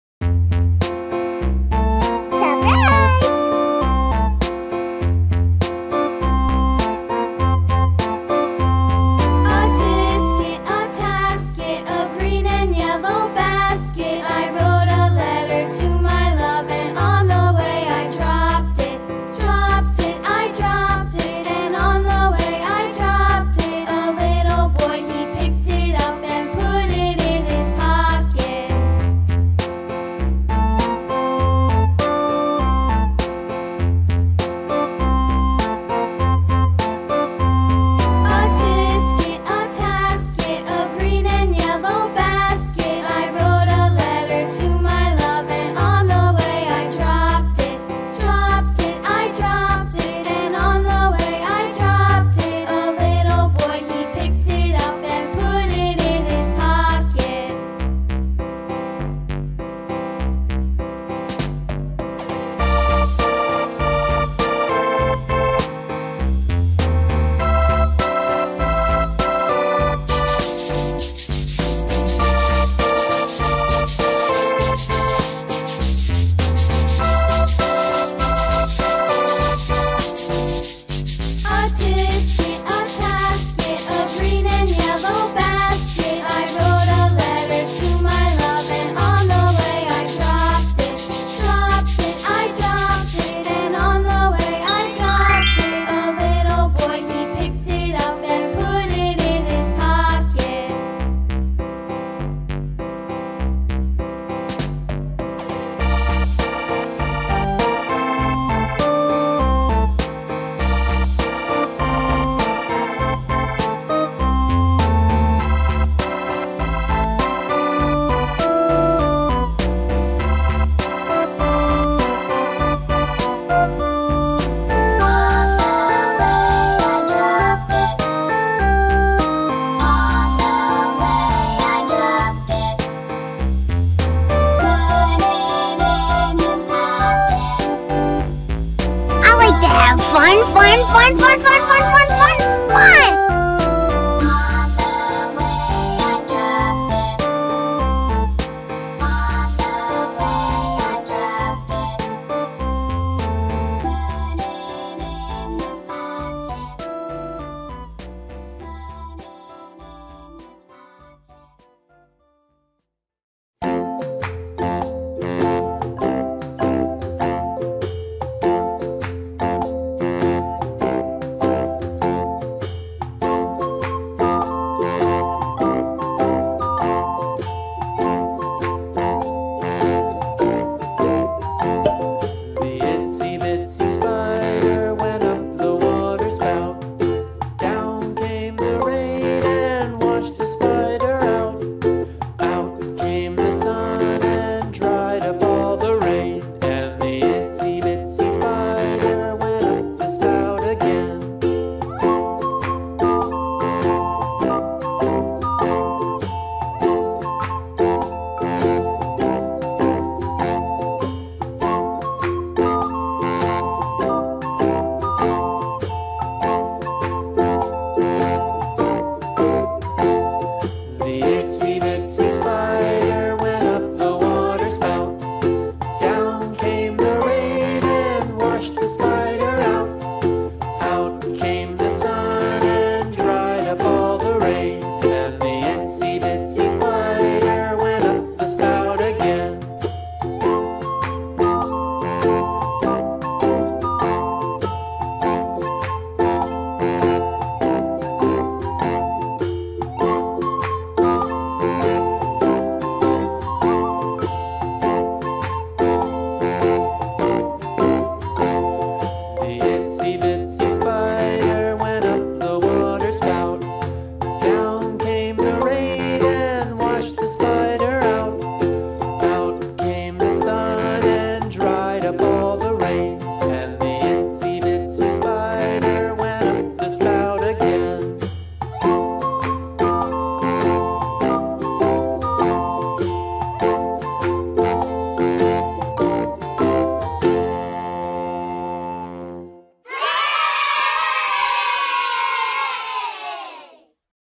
Nail Polish | Toenails Drawing and Painting for Kids, Toddlers ♡ sunny kids art ♡
hi world! let's learn how to draw Toes and paint Toenails with Neon Nail Polish!